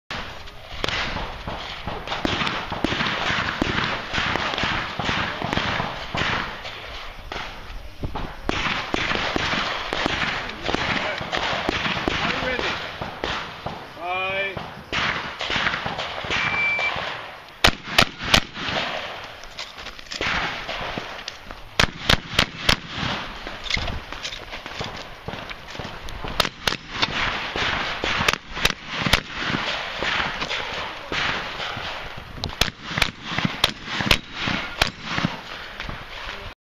Raine Peltokoski - Shotgun World Shoot 2015 Italy Stage 5
One of the 6 stages where Raine won all divisions.